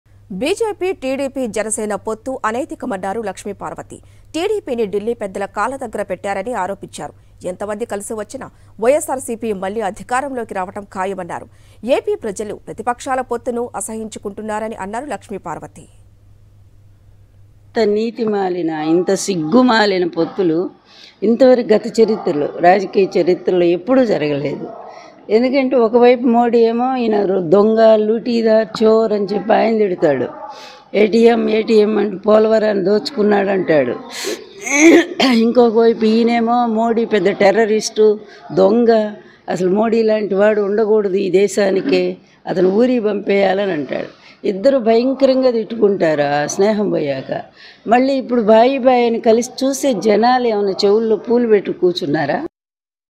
Lakshmi Parvathi Comments On TDP Janasena & BJP Alliance | Chandrababu
Video Description: Explore the political landscape as Lakshmi Parvathi shares her comments on the alliance between TDP, Janasena, and BJP. Gain insights into her perspectives on this strategic collaboration and its potential implications for Andhra Pradesh. Delve into the nuances of Parvathi's remarks, offering a closer look at the dynamics between these political entities. Stay informed on the unfolding developments and the impact this alliance might have on the upcoming political scenarios in the region. Uncover the complexities of alliances and political strategies through the lens of Lakshmi Parvathi's commentary.